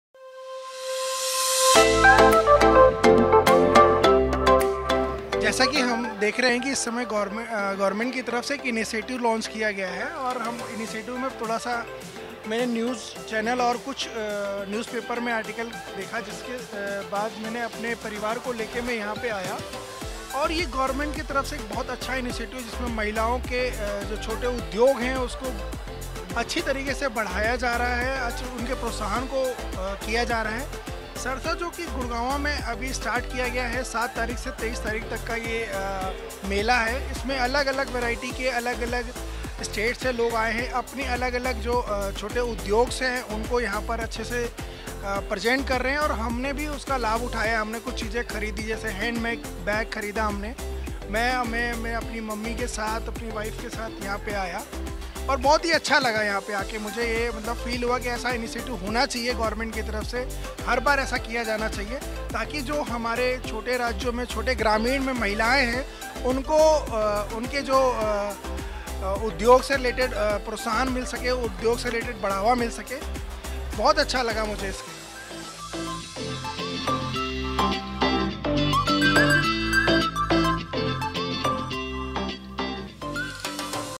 आगंतुकों ने सरस आजीविका मेला को ग्रामीण महिला कारीगरों के लिए बेहद फायदेमंद बताया।
सरस आजीविका मेला, गुरुग्राम में आए आगंतुक ने सरकार की इस योजना की प्रशंसा करते हुए इस योजना को महिला कारीगरों के लिए बेहद महत्वपूर्ण बताया।